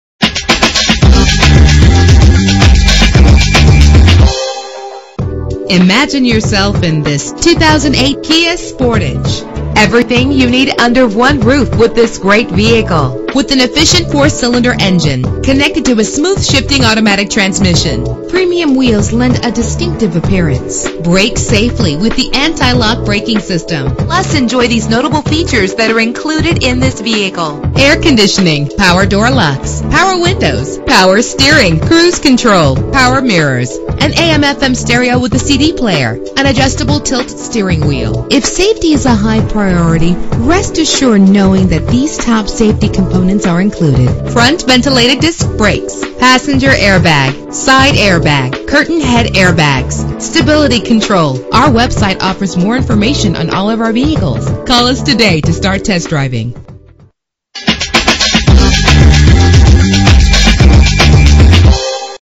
2008 Kia Sportage - Nashua NH
Year: 2008
Make: Kia
Model: Sportage
Trim: LX
Engine: 2.0 liter inline 4 cylinder 16 valve
Transmission: Automatic
Color: Gold
Mileage: 70804
Address:
280-300 Amherst St